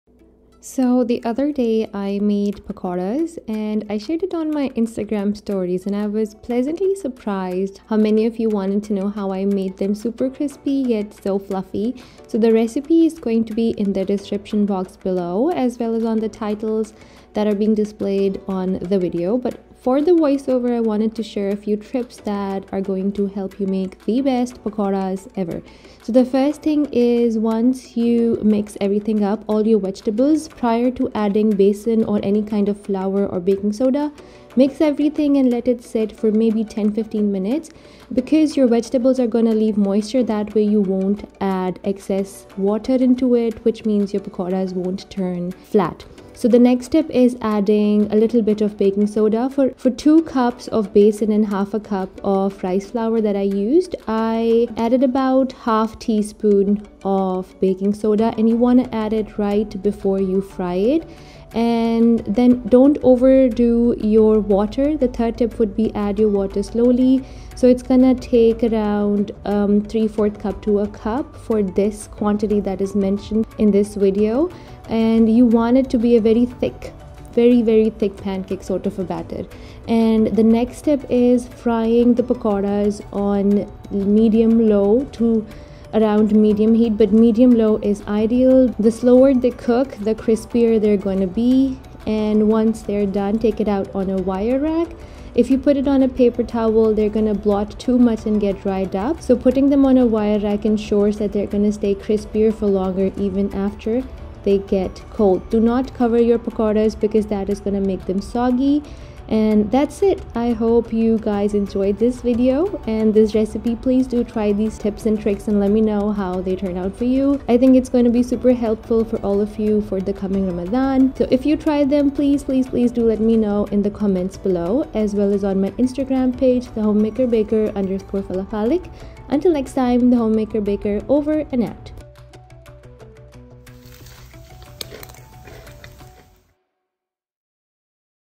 How To Make Perfect Crispy Fluffy Pakoras
Ramadan is around the corner and pakoras are a staple at Iftar time. We all have our own recipes and iterations for these well-loved vegetable fritters. That’s why I mainly wanted to focus on the tips and tricks that ensure your pakoras are fluffy yet super crispy. If you are new to making pakoras, worry not. I have also added my basic recipe in this video which you can of course customize as per your liking =)

Ingredients:
1 larger or 10-12 small Potatoes - Thinly chopped
2-3 small Onions - Thinly Chopped
1.5 tbsp Coriander Seeds - Roasted & Ground
1 tbsp Cumin Seeds - Roasted & Ground
1 tsp Red Chilli Powder
1/4 tsp Turmeric Powder
1 tsp Chaat Masala or Garam Masala
1/4 cup Chopped Fresh Coriander
1 tbsp Fenugreek Leaves
2 cup Chickpea Flour (Besan)
1/2 cup Rice Flour - Optional but makes the pakoras super crispy!
1/2 tsp Baking Soda
1/2 tsp or Taste Salt

NOTE:
You may also add 1/2 tsp Red Chilli Flakes AND/OR chopped green chillies to make them a bit spicy. Also, you may use other vegetables like carrots, spinach etc.

TIPS for making the perfect Pakoras

1. Mix all the ingredients except for flours & baking soda and let them sit for at least 10mins. This will ensure you don’t accidentally add too much water which can flatten the pakoras.
2. Add in some rice flour to make the pakoras super crispy.
3. Add baking soda to make fluffy pakoras
4. Fry on medium-low heat, the slower and longer they cook, the crispier they will turn out.
5. Take them out on a wire rack so the steam can vent through, keep the pakoras crispier for longer
6. Do not cover your pakoras as the steam will make them soggy.